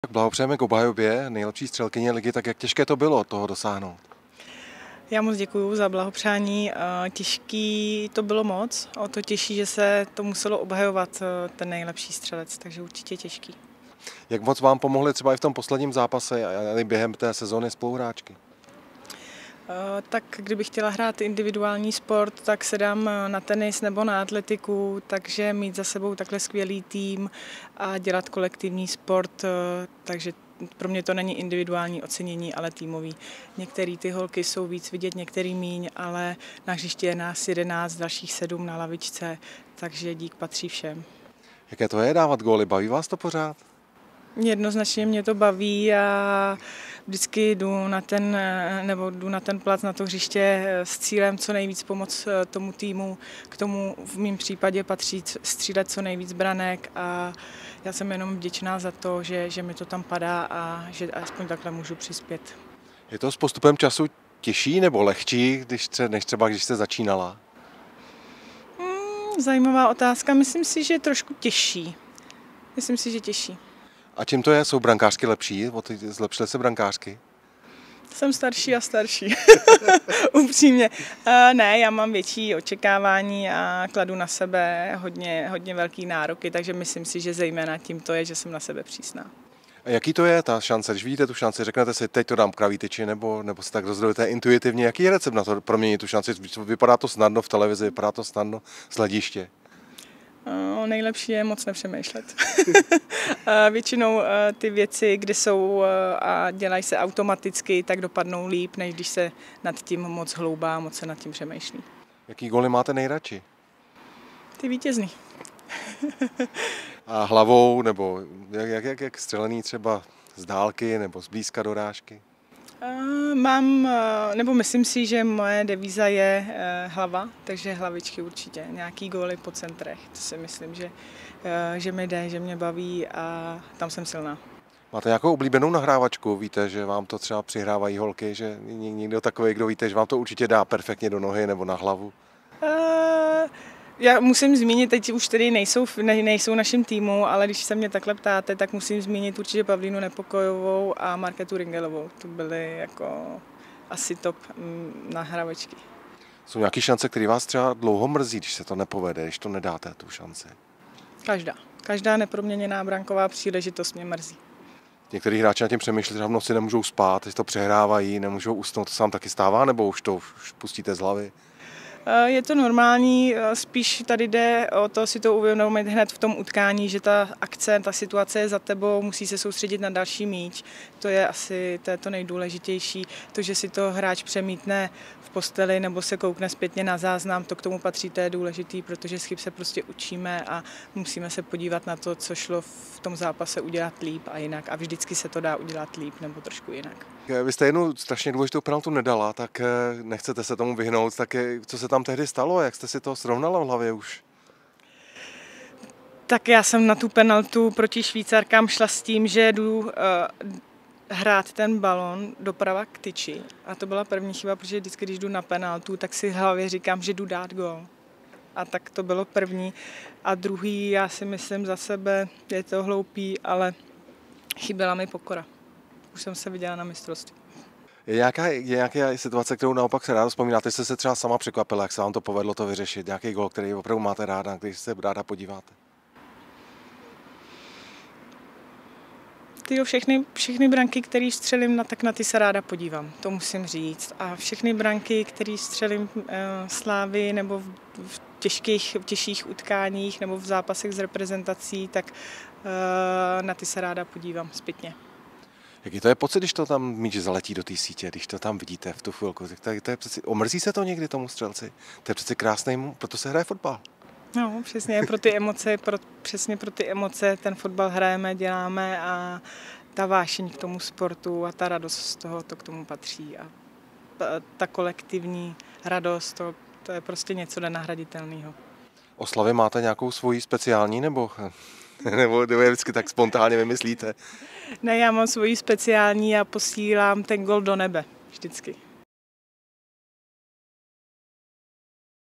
Lucie Martínková má nejraději vítězné góly
Útočnice pražské Sparty Lucie Martínková dala v minulé sezoně dvacet čtyři prvoligových gólů a obhájila tak titul nejlepší střelkyně soutěže.